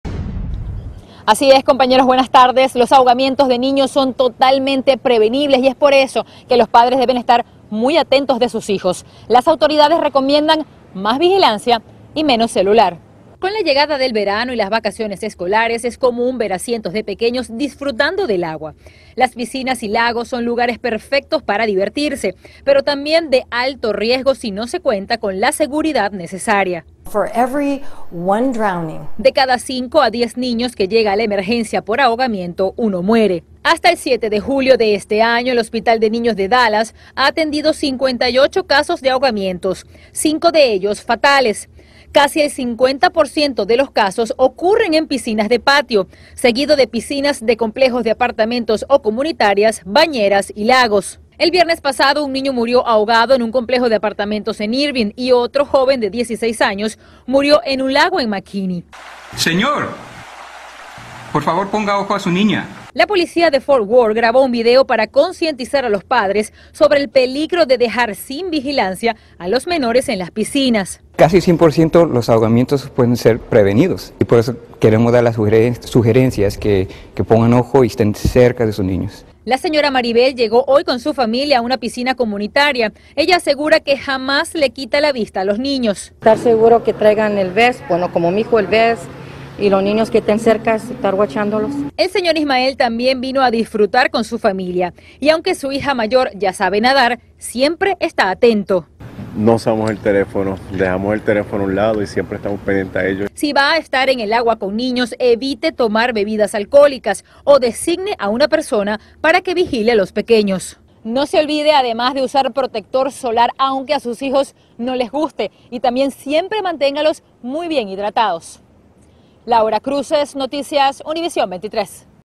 Atención padres, los ahogamientos de menores son totalmente prevenibles y estas son las recomendacio
La llegada del verano y las vacaciones escolares hacen que las piscinas, lagos y playas sean los escenarios preferidos de los pequeños, sin embargo, es necesario que ellos estén siempre vigilados para evitar tragedias. La policía de Fort Worth lanzó una campaña para que los papás tomen conciencia y sigan simples pasos como no ingerir bebidas alcohólicas y no usar el celular mientras se está al cuidado de un infante.

{url}